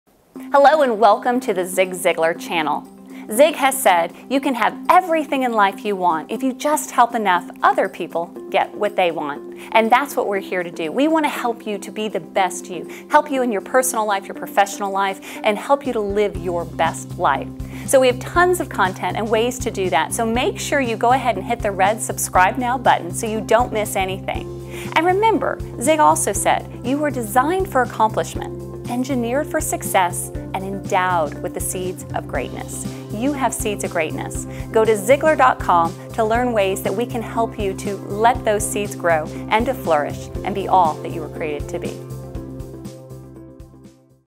Welcome to the Official Zig Ziglar YouTube Channel!
Welcome to the OFFICIAL YouTube channel of Ziglar.com!

Click "Subscribe" to get the best Ziglar content on the web, including the Ziglar show, countless tutorials and presentations by Ziglar Certified Trainers, and some of the best moments from speeches given by Zig himself.